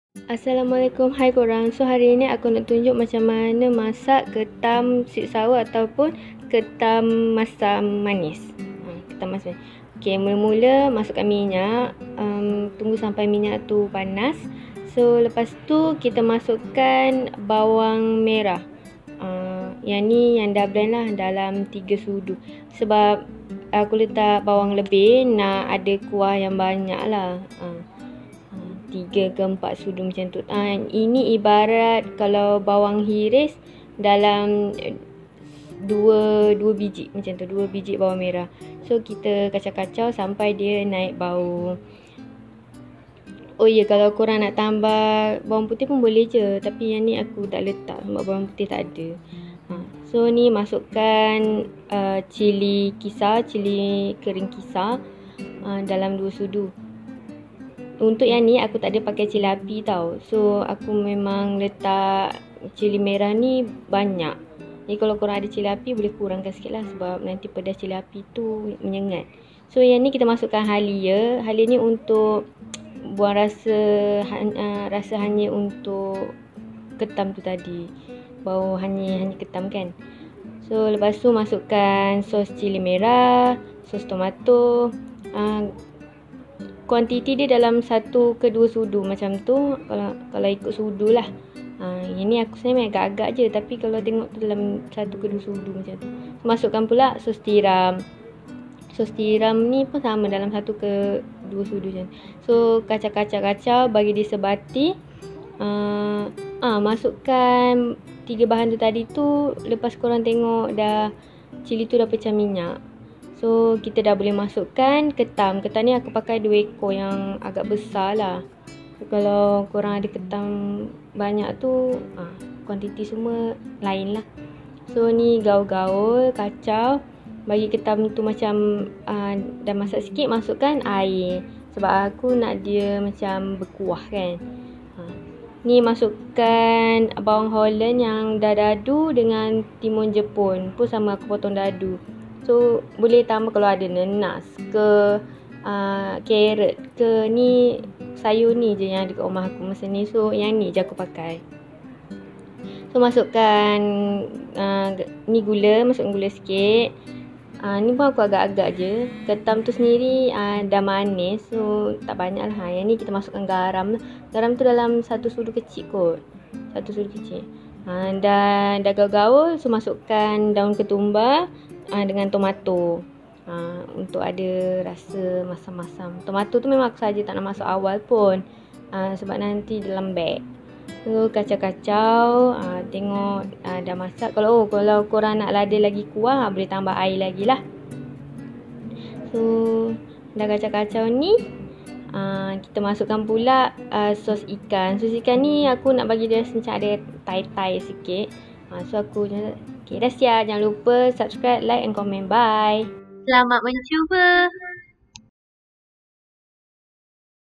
KETAM MASAM MANIS / SWEET SOUR senang je!
Bahan yang aku guna mesti ada dalam peti ais korang juga.

BAHAN-BAHAN
2 ekor ketam (belah dua)
2 sudu bawang merah blend / 2 biji bawang merah untuk hiris
2 sudu cili kisar
2 sudu sos cili
2 sudu sos tomato
2 sudu sos tiram
1 sudu sos ikan
Sayur (timun, bawang holand, tomato, cili merah, daun ketumbar)
Garam dan Gula
Air

p/s: tiada yang salah dalam masakan, cara je yang berbeza. Selamat mencuba!